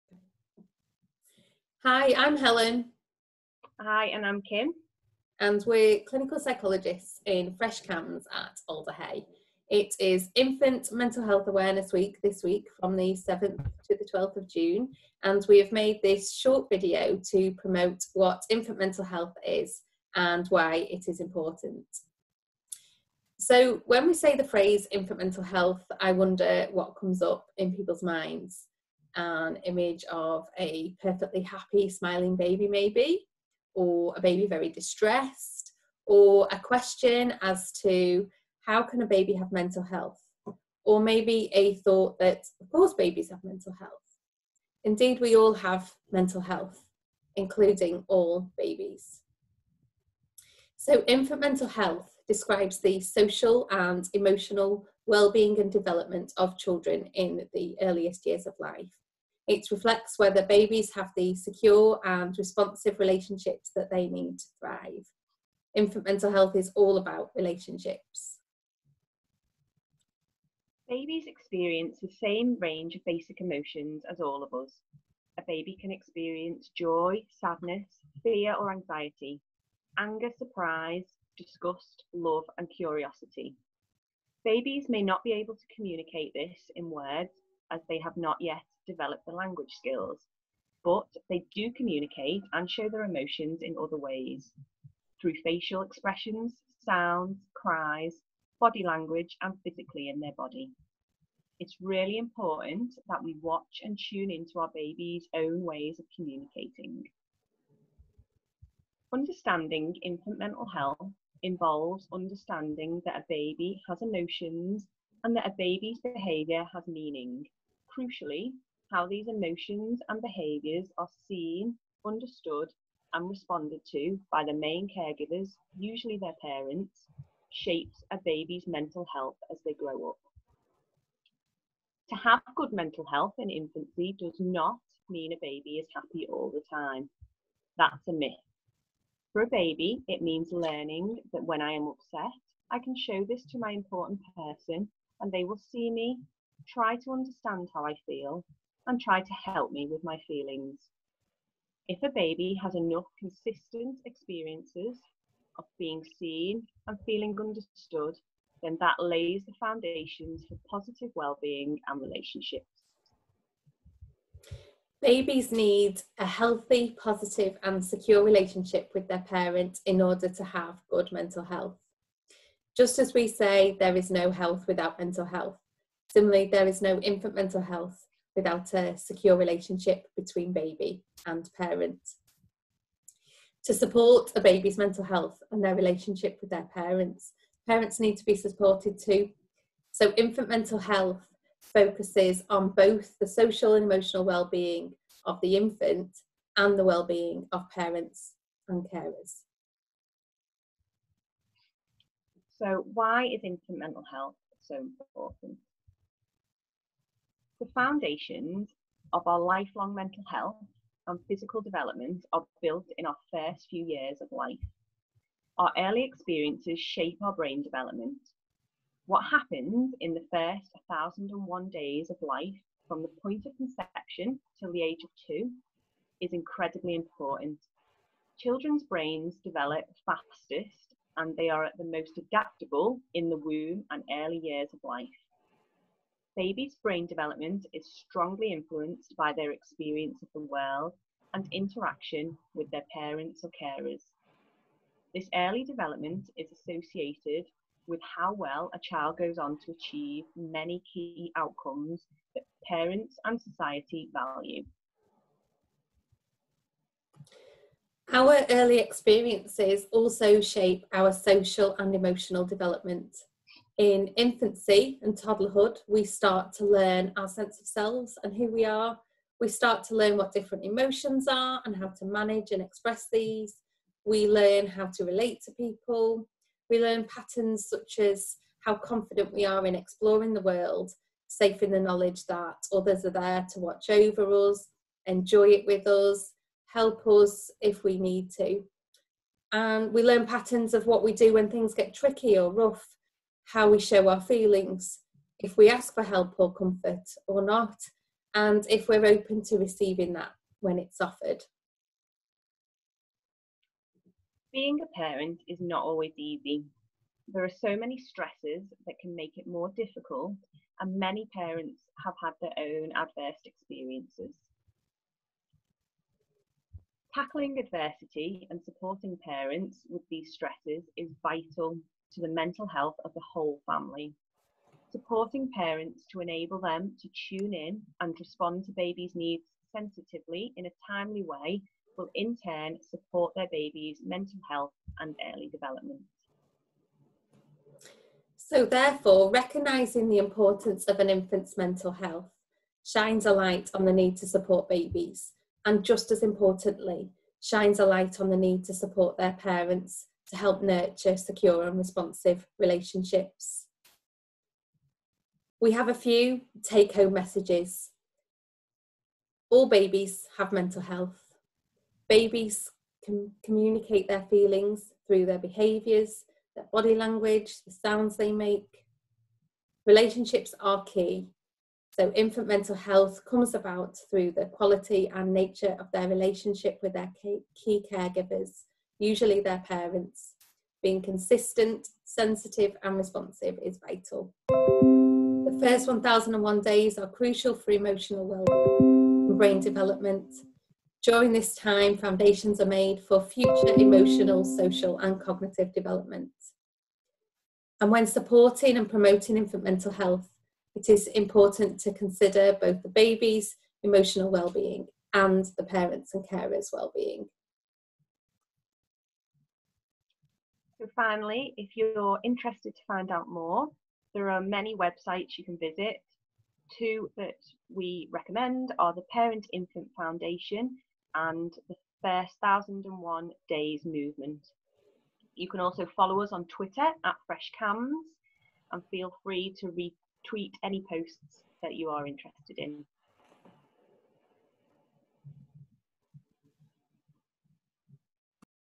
What is Infant Mental Health?
Dr Helen Honor and Dr Kim Embra (Clinical Psychologists) in Fresh CAMHS have made a short presentation promoting what infant mental health is and why it is important.